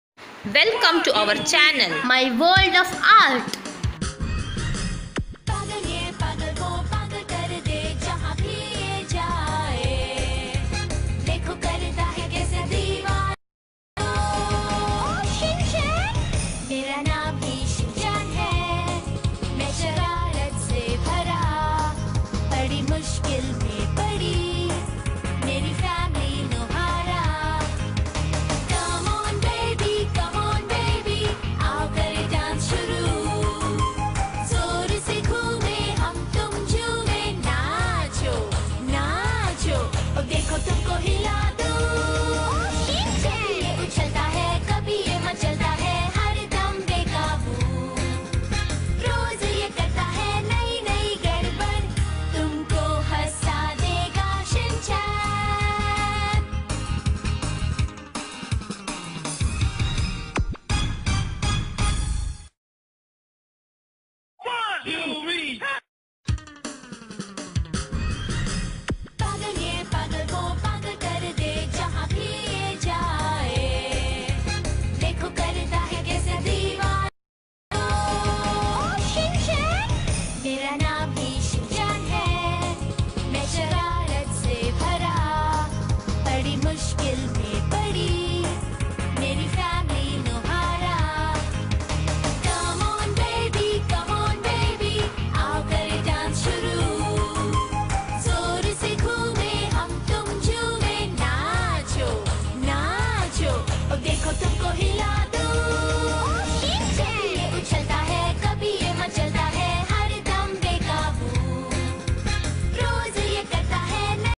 Easy Shinchan Nohara pencil colour drawing by Sharanya
Today's art is by Sharanya (Ramya's daughter)

Materials used :

1. Drawing book
2. Pencil colours
3. Pencil
4. Black sketch pen